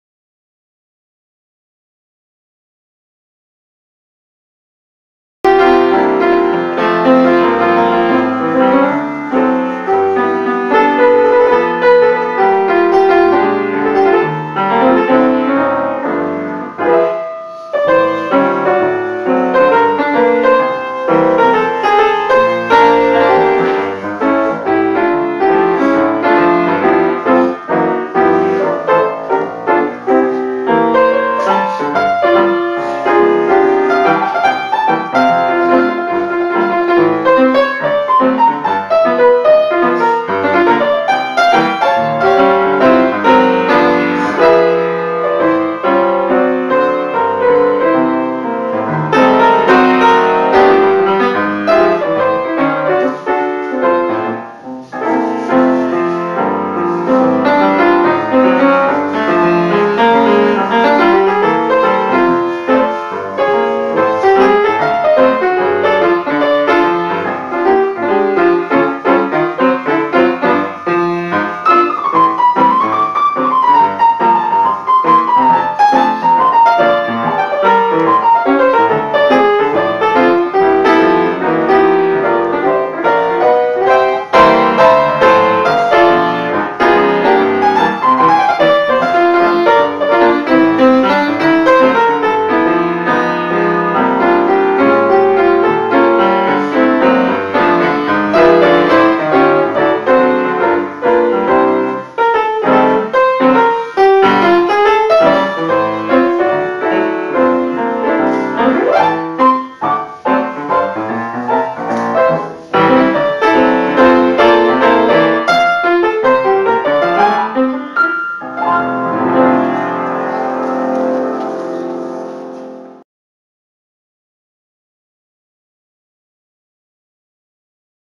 Gershwin-Pianist Mike Benjamin
Gershwin played by pianist Mike Benjamin on the Mammoth Piano VCG